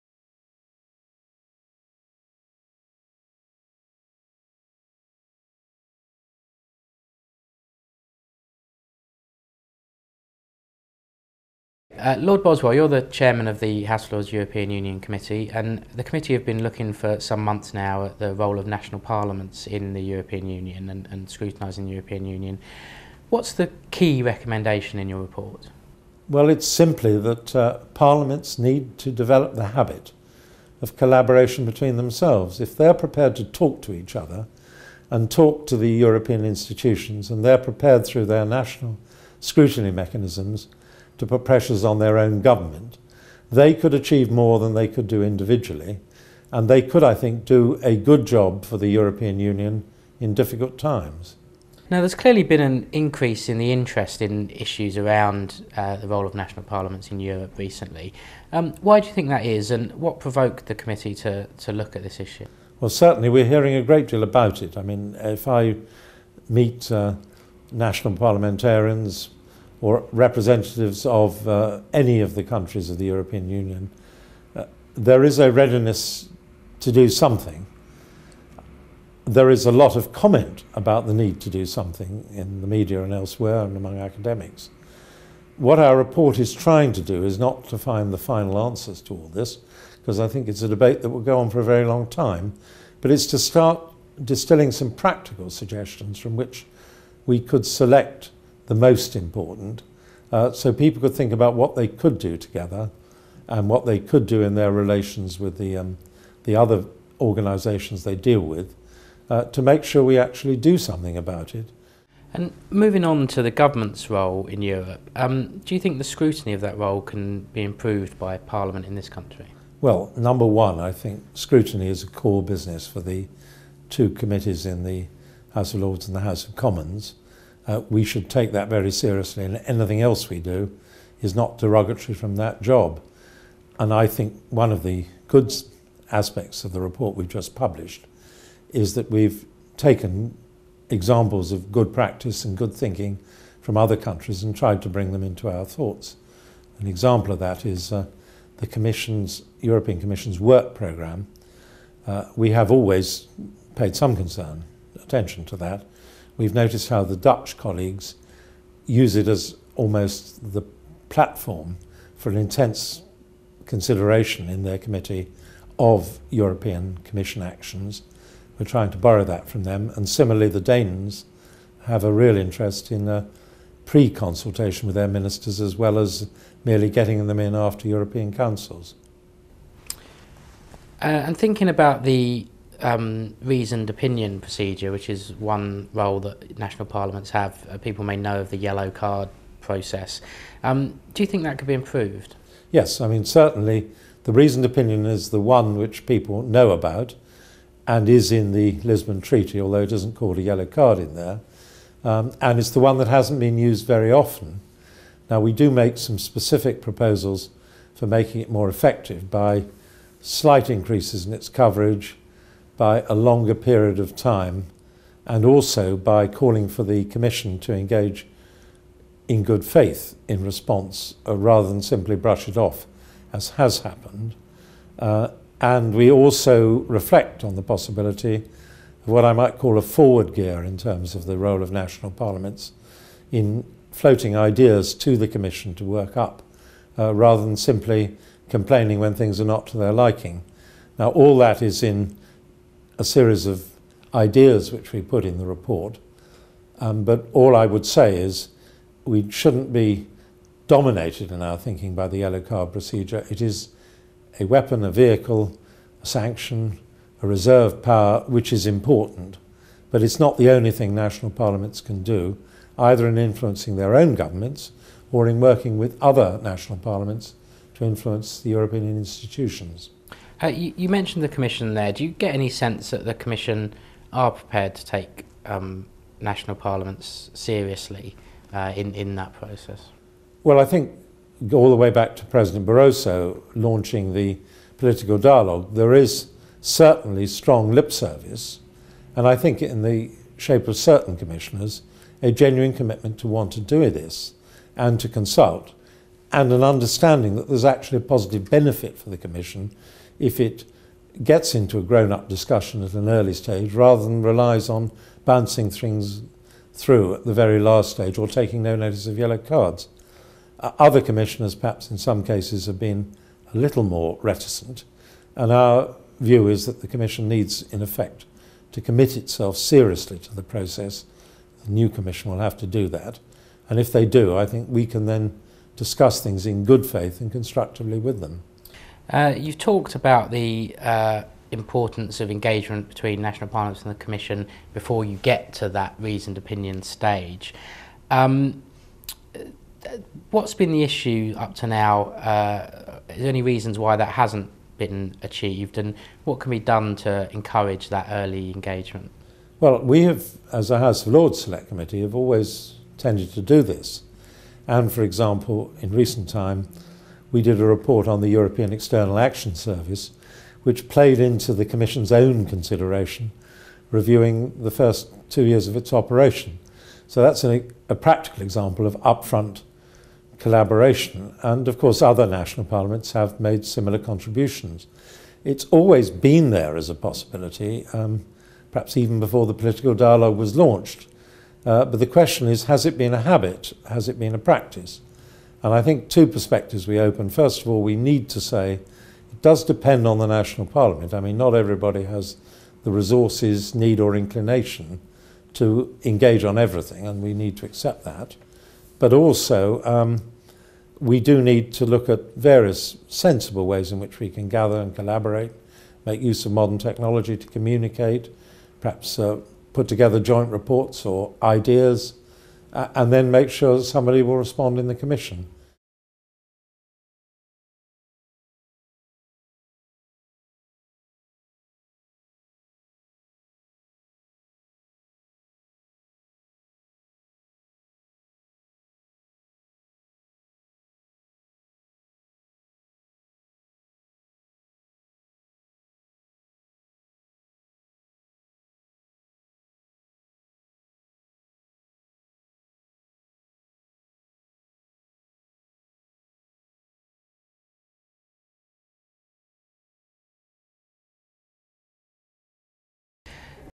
Role of National Parliaments in the EU | EU Select Committee | House of Lords
Lord Boswell, Chairman of the House of Lords EU Select Committee, talks about the report into the role of national parliaments in the European Union.

For more information about the Committee and a full copy of the report,